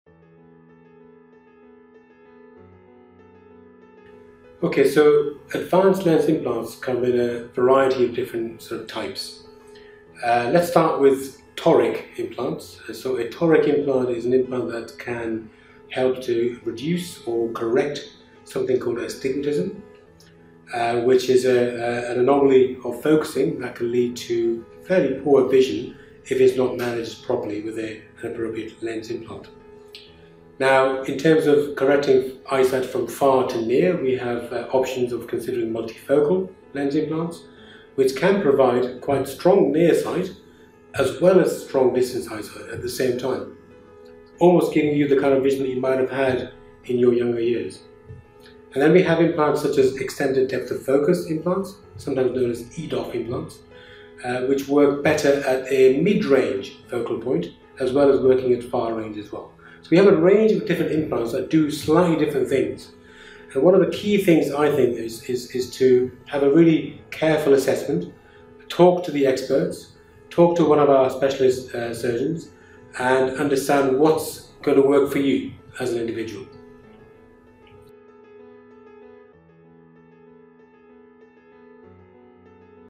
What are the differences between monofocal and advanced lens implants?
Our consultants explain the difference between monofocal and advanced lens implants for your cataract surgery? Learn about the key differences from how each lens works to how they affect your vision at various distances.

At Optegra, we’re here to guide you through your lens options, whether you're considering monofocal, multifocal, or EDOF lenses, so you can choose what’s right for your lifestyle and vision goals.